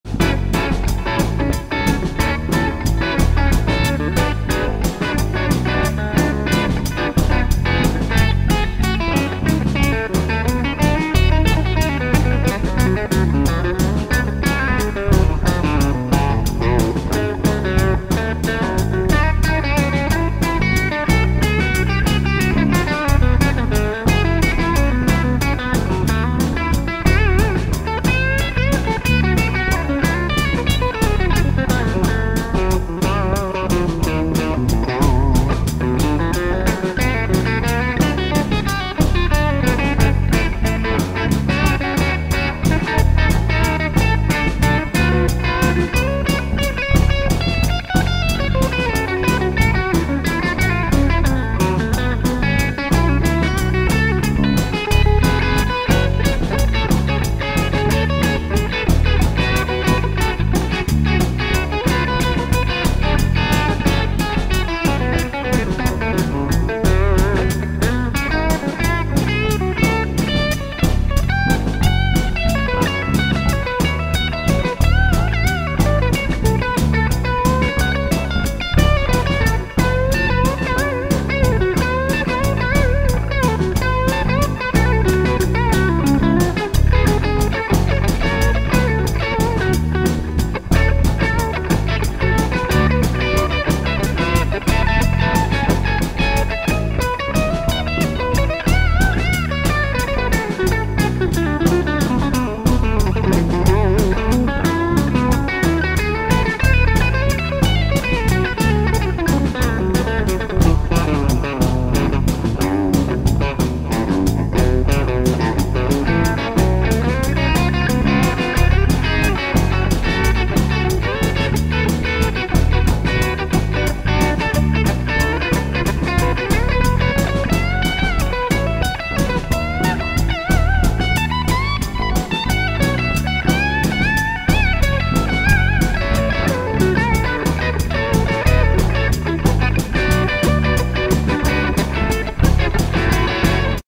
african 6/8 rhythm | guitar jam
this is guitar improvisational music on an african 6/8 rhythm / groove